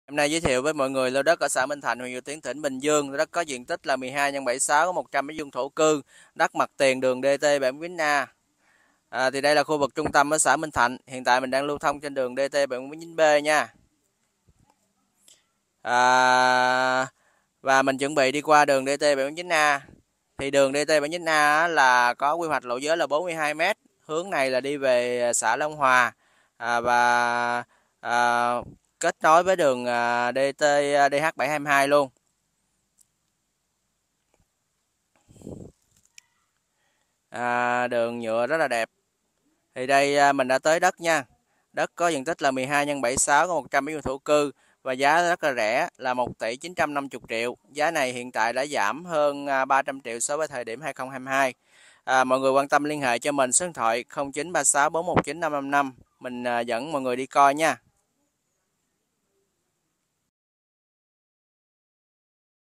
Đất Minh Thạnh Dầu Tiếng MT DT749A, KDC. Dt 12x76, 100m2 thổ cư. Giá 1,95 tỷ. LH 0936419555 Mr Định
CHÚC CÁC ANH CHỊ 1 NGÀY MỚI VUI VẺ !
© Bản quyền thuộc về Tuong Van Real
© Copyright by Tuong Van Production ☞ Do not Reup